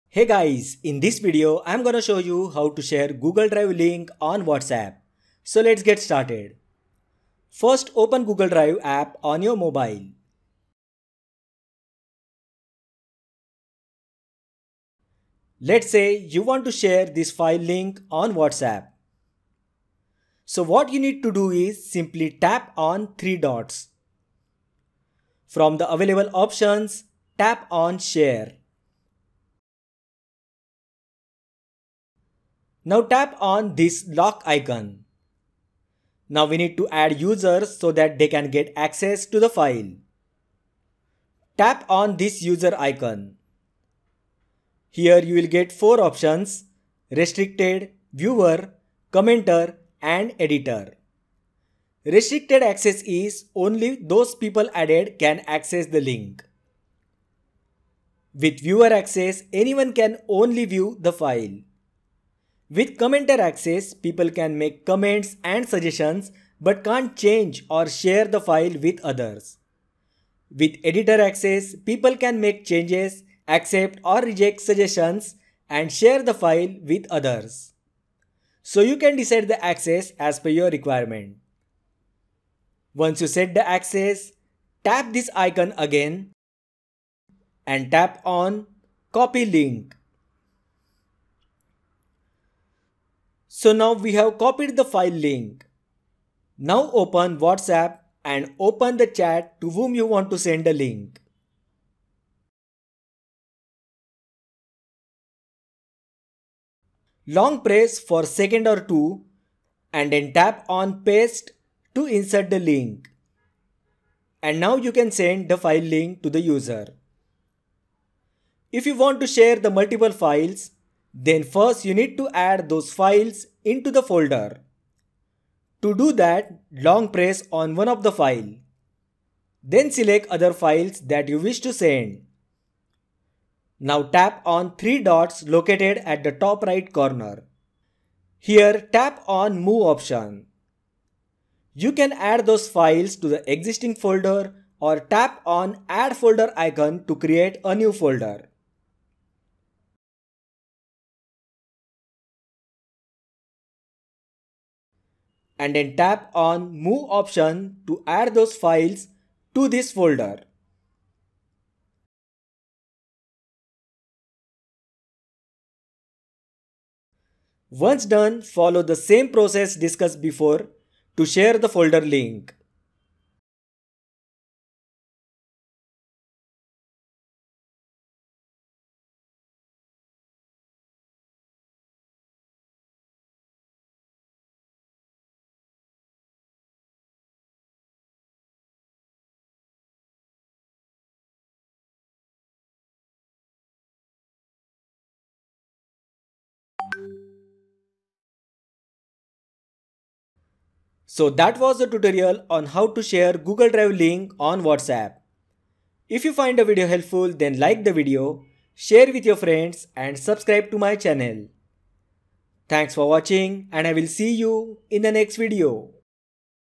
How To Share Google Drive Link On Whatsapp | Share Google Drive Files On Whatsapp
How To Share Google Drive Link On Whatsapp. Video is about how to share Google drive files on Whatsapp on mobile. If you are not sure about how to share files on google drive or how to share google drive folder link on whatsapp, then this video is for you. This step by step how to tutorial will help you to share google drive shareable link on whatsapp android and iPhone. To do this you should have Google drive app installed on your mobile device. The process is quick and straight forward and that way you can share google drive link on Whatsapp and then send the message to your friends.

►► Recommended Videos -